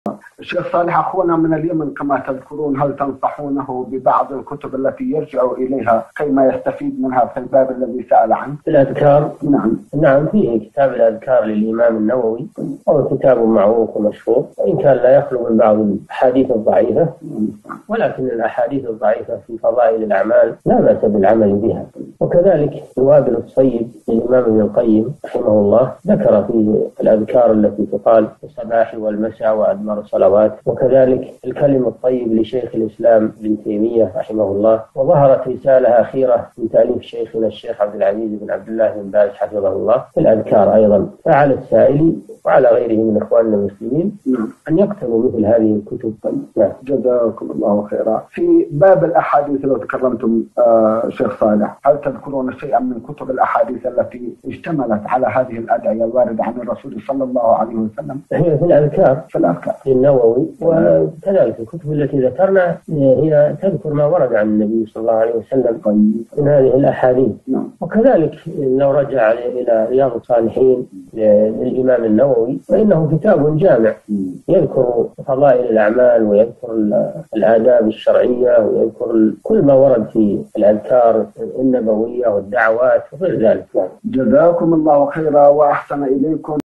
من أهم الكتب في الأذكار - الشيخ صالح بن فوزان الفوزان
نص السؤال:
من أهم الكتب في الأذكار
السيرة الذاتية :
للاشتراك في قناة الشيخ صالح الفوزان

رئيس الفريق
لمشروع كبار العلماء